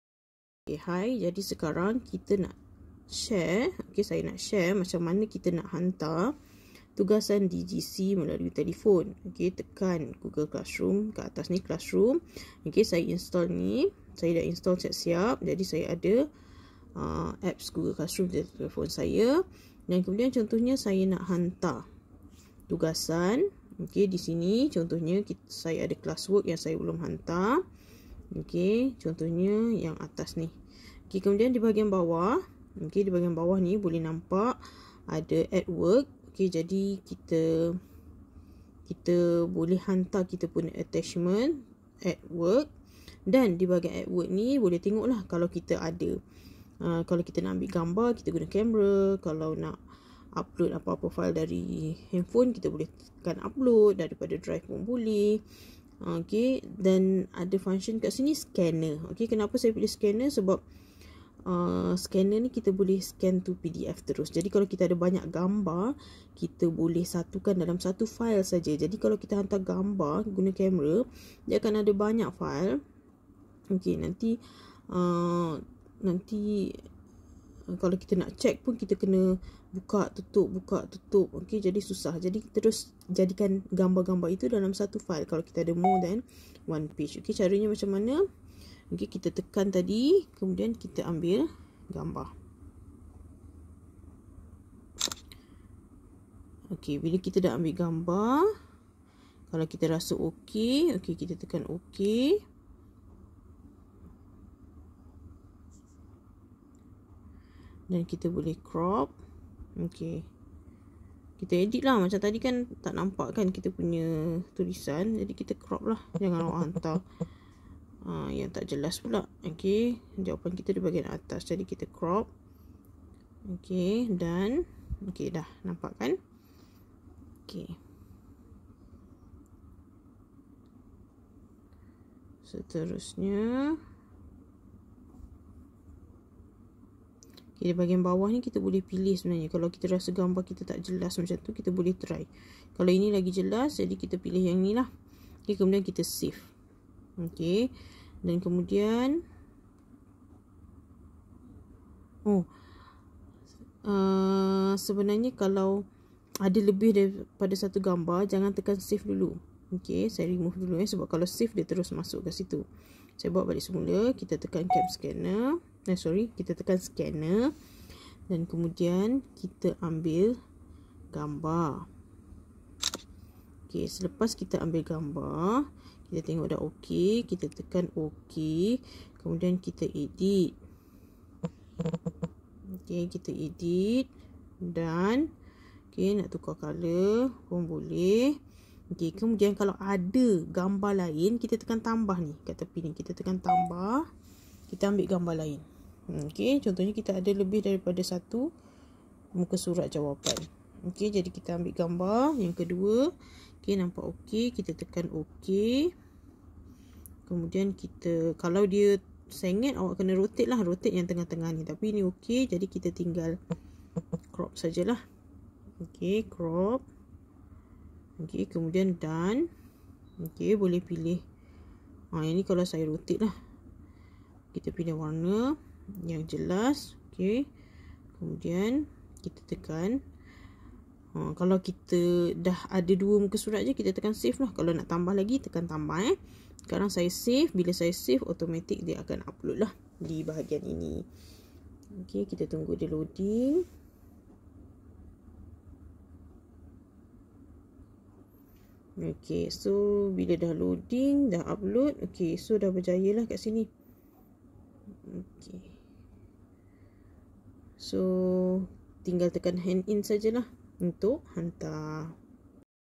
Hantar jawapan ke GC menggunakan scanner dalam Apps GC.
Kalau hantar guna image..terlalu banyak file yg perlu dibuka untuk melihat kesemua jawapan murid (jika lebih dari satu muka etc).

Jika guna format PDF, hanya buka satu file sahaja, semua gambar jawapan boleh disatukan dalam satu file pdf.

Sebelum tekan 'save', boleh rename dulu file tersebut dengan nama sendiri. Untuk rename, tekan nama file di bahagian atas.

Dengan menamakan semula file dengan nama murid, akan memudahkan guru untuk semak. tqsm.